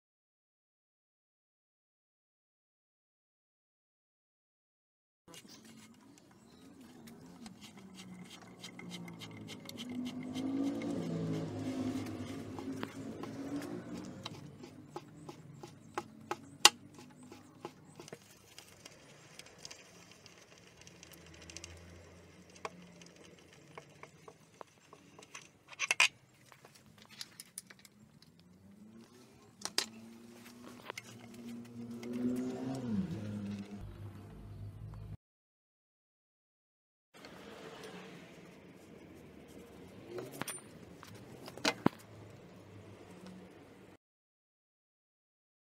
C-Bear installation on Lotto-Soudal ladies TT
Lotto-Soudal ladies team mechanic showed  C-bear FitNForget InstallnRide zero-creak bottom bracket with ceramic bearings, onto the team TT bikes.

6 team riders going on to Rio with new Ridley Fenix will be with C-BEAR PF41: